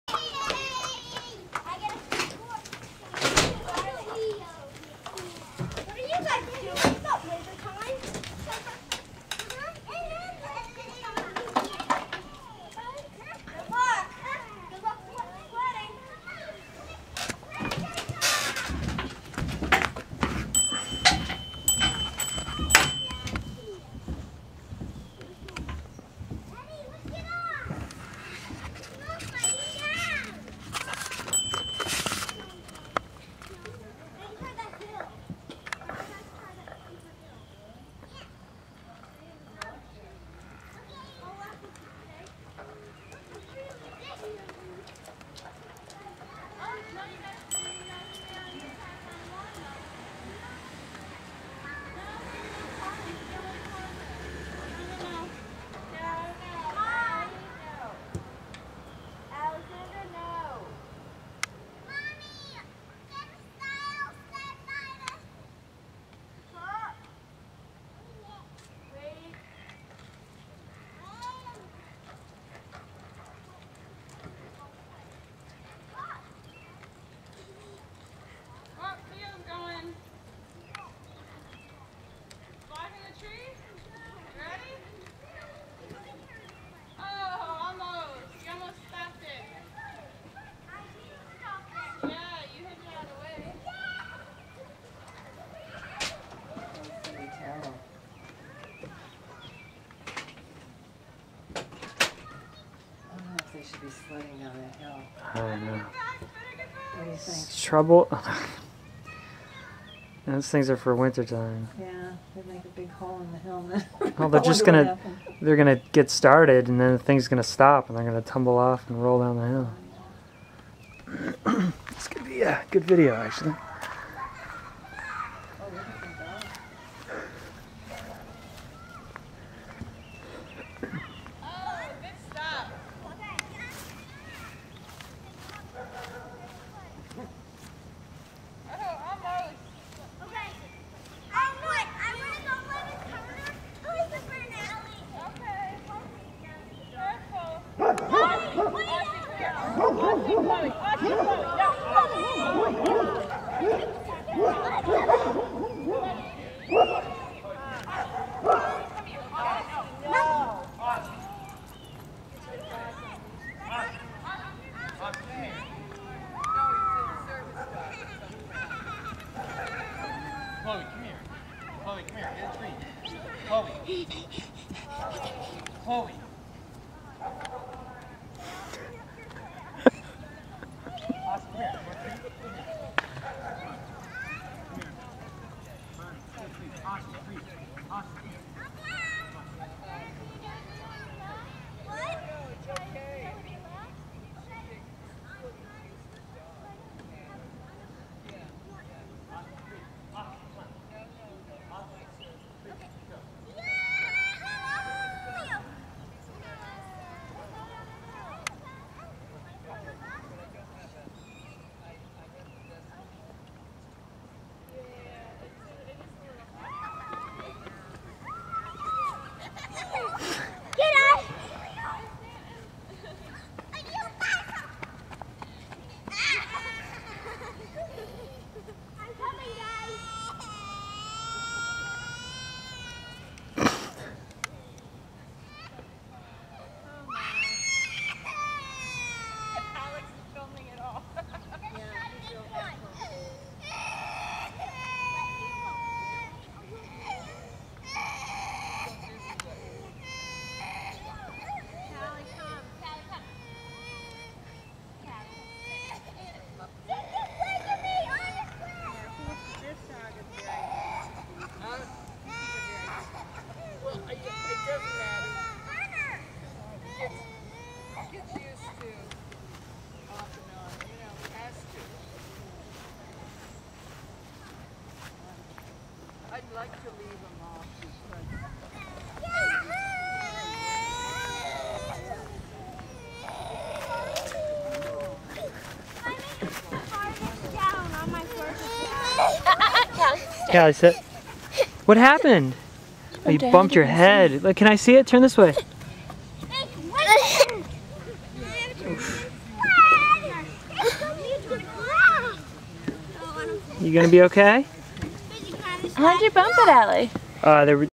Fun in Maine.
Sittin' on the porch watching the kids play... and bump heads while sledding on the lawn.